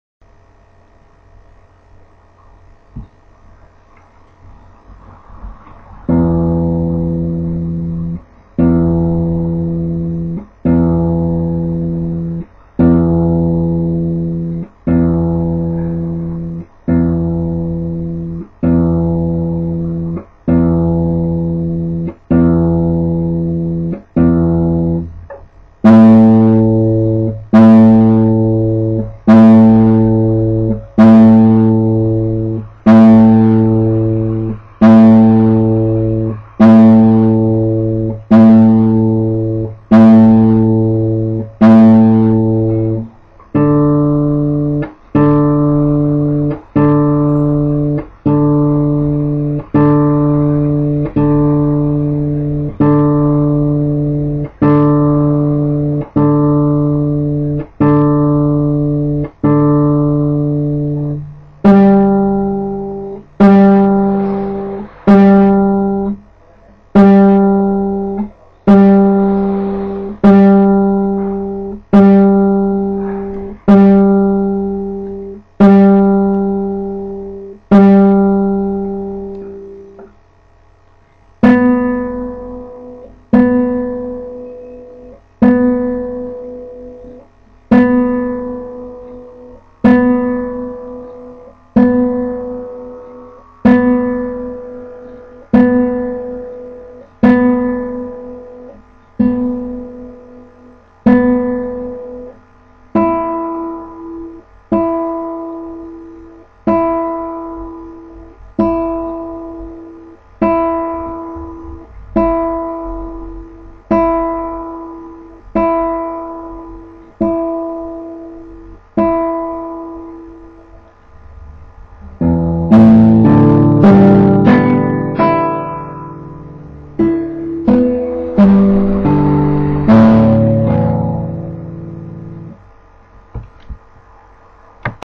Kitarra aurrindu
Gitarra/Kitarra entzumenaren bidez aurrindu /afinatu/ tonuan jarri/ gozatu
Behin baino gehiagotan egitea komeni da, batez ere kordak jarri berriak direnean.